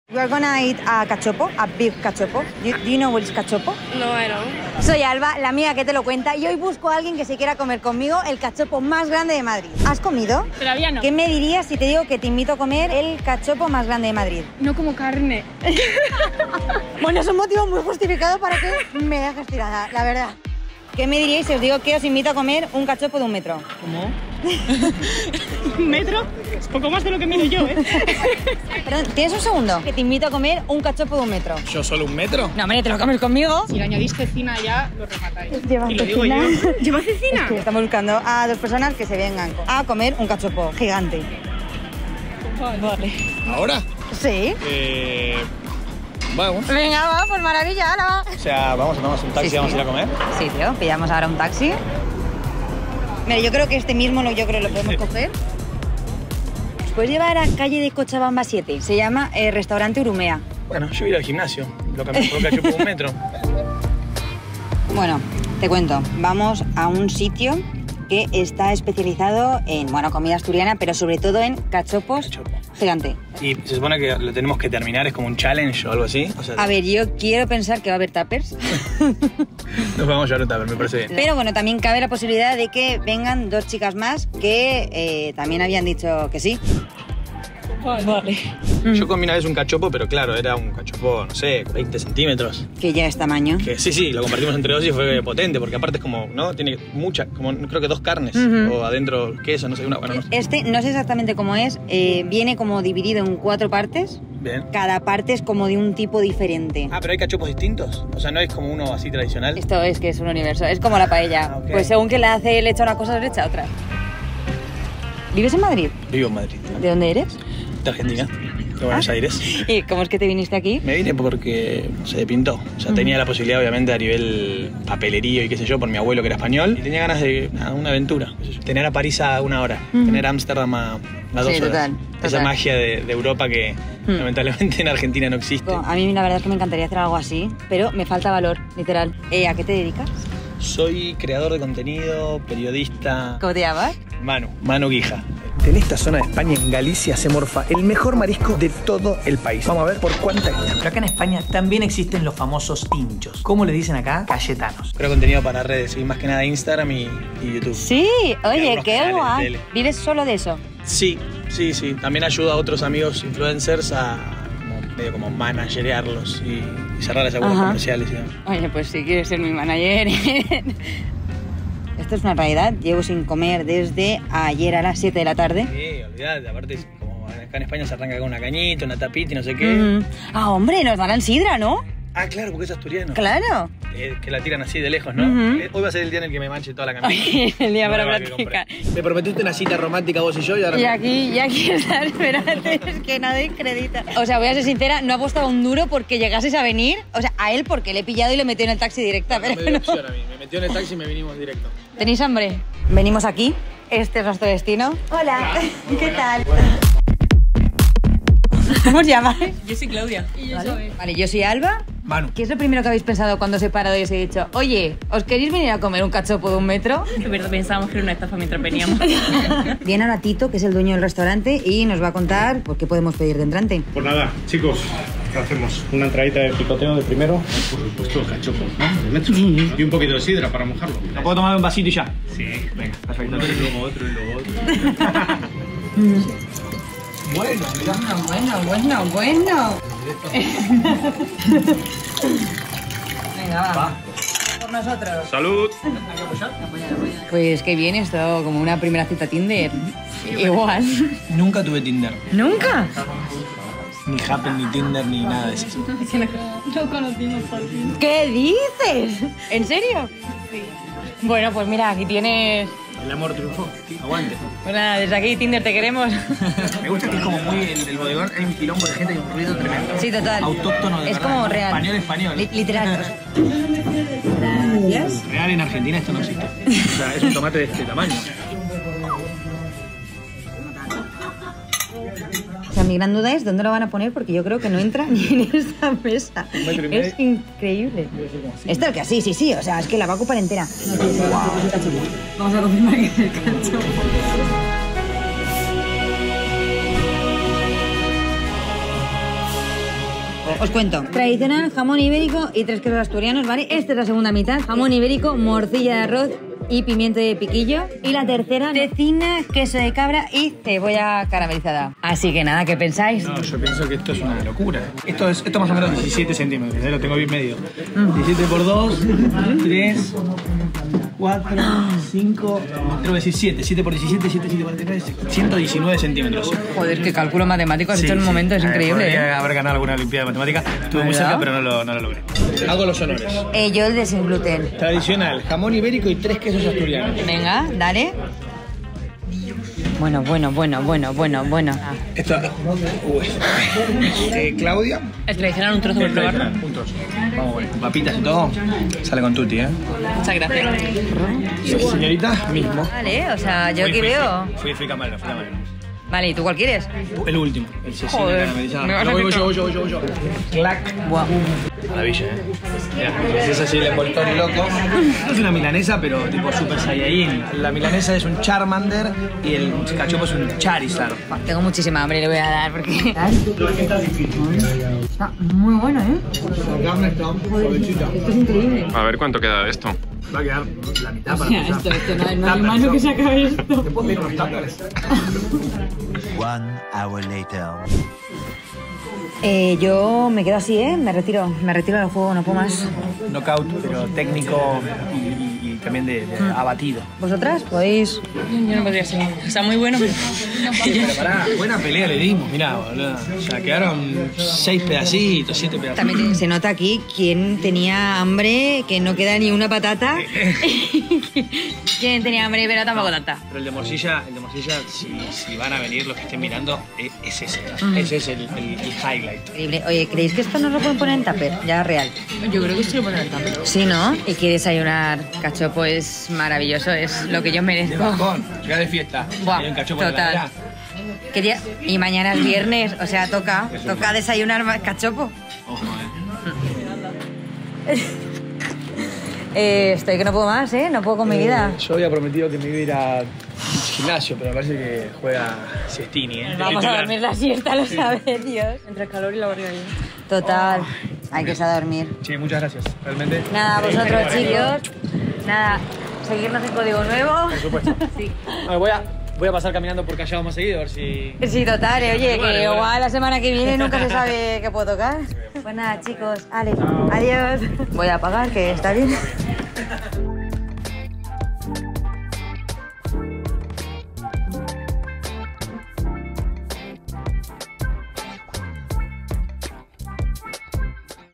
El CACHOPO más GRANDE de MADRID | Invitando a un random
-¿Qué me dirías si te dijera que te invito a comer el cachopo más grande de Madrid?
-Que no te conozco de nada.
-Ya ¿pero vendrías o qué?

Alba Jiménez buscas a personas de la calle para invitarlas a los planes más locos de todo el país.